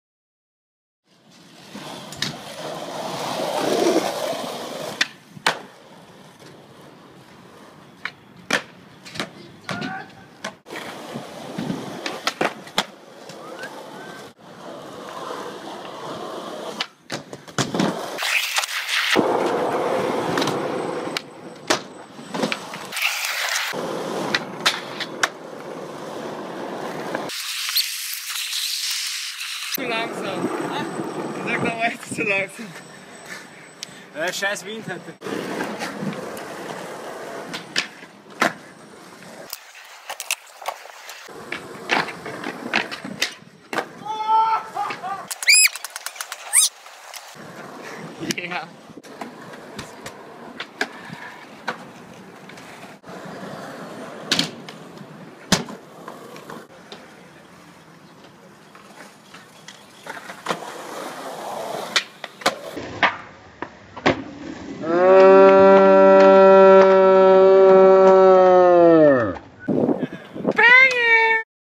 Skatepark Längenfeldgasse iPhone Footy - Skateboarding Vienna
Clemens, Robert and David beim Skateboardtraining im Skatepark in der Längenfeldgasse in Wien. Gefilmt mit einem iPhone 5.